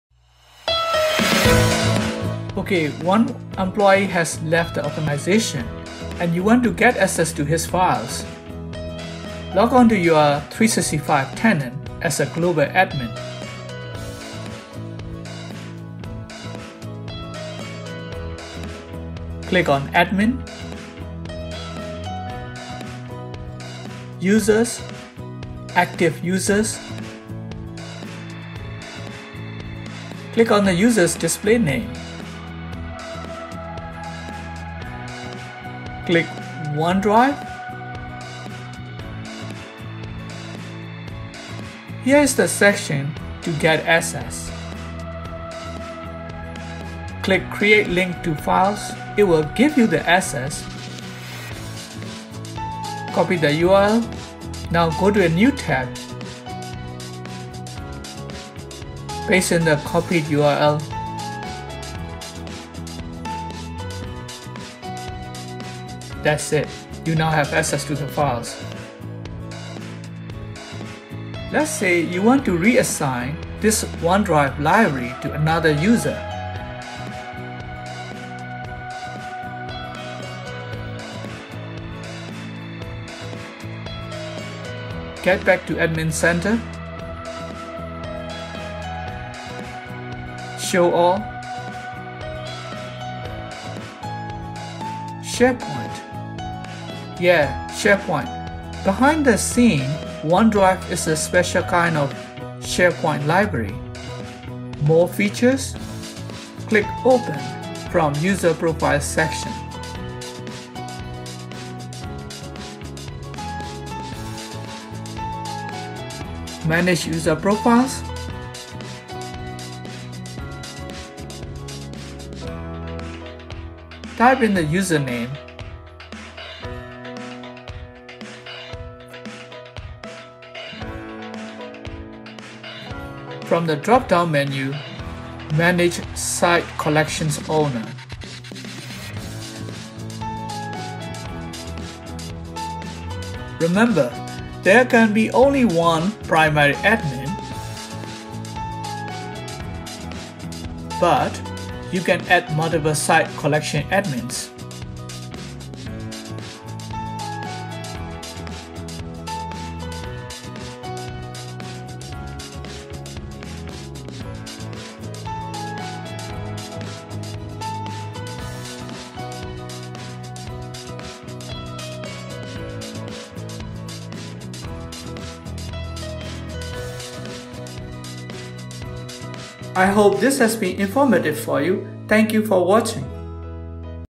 365 Admin: Getting access of ex-employee's OneDrive files
- Getting direct URL to OneDrive library of employees
- Taking control of OneDrive of Ex-employees
- Re-assigning (or giving access of) OneDrive library to another user